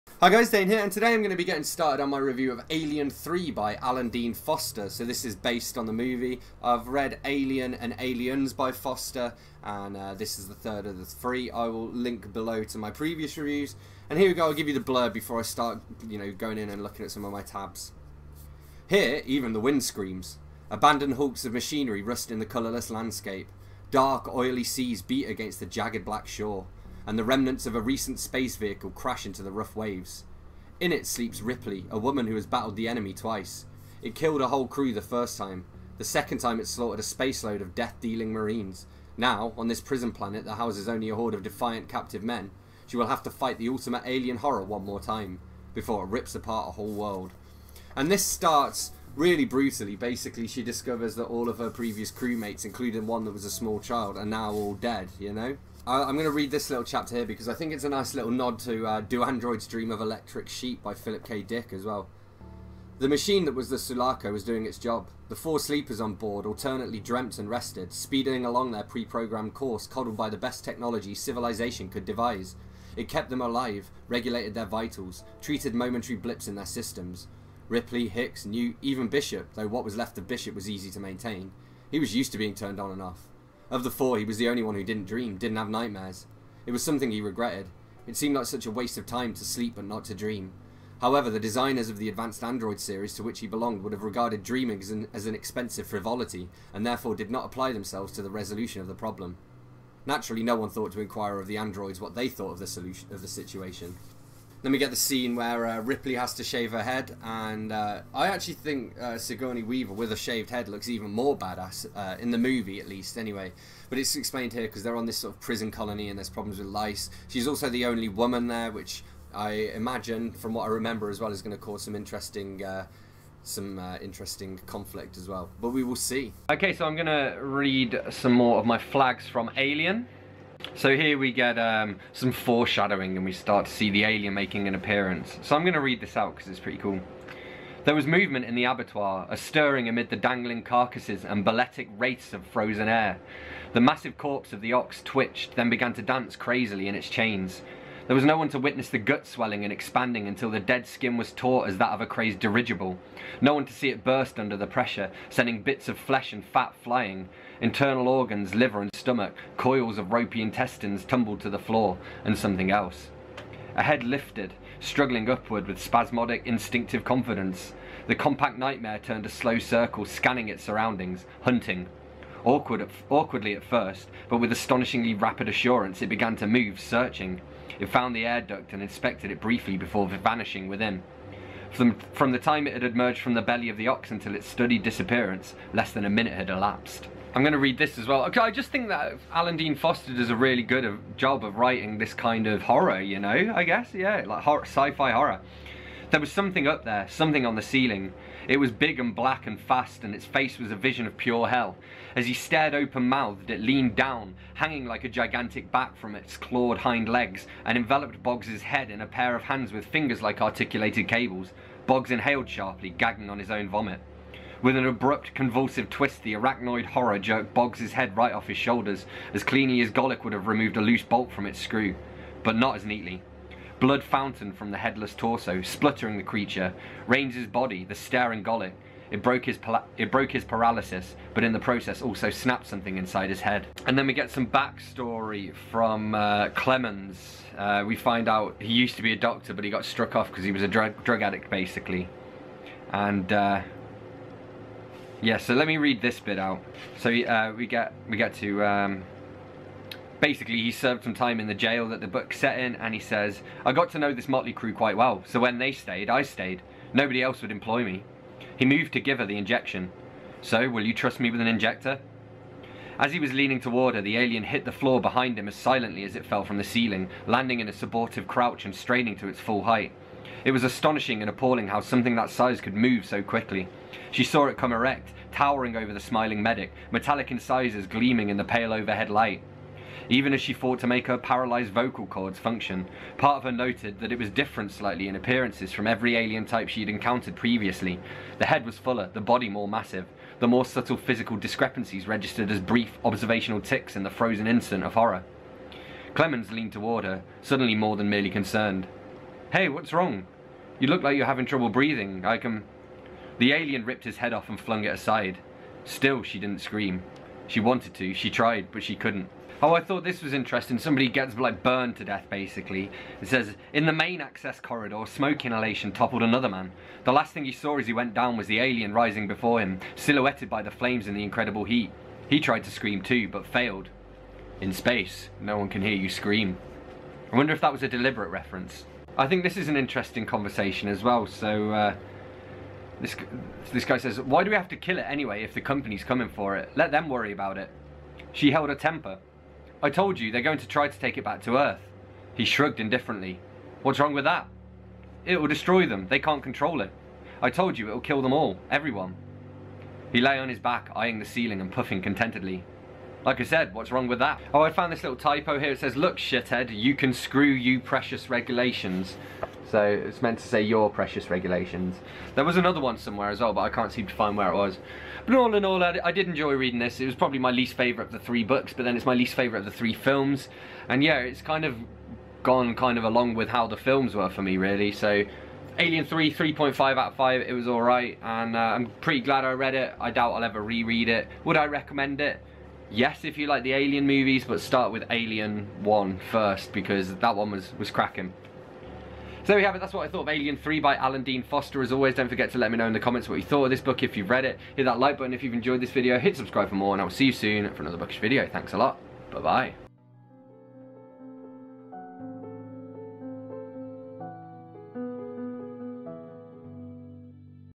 Alan Dean Foster - Alien 3 [REVIEW/DISCUSSION]
Here goes!

BOOKS MENTIONED:
Alan Dean Foster - Alien
Alan Dean Foster - Aliens
Alan Dean Foster - Alien 3
Philip K. Dick - Do Androids Dream of Electric Sheep?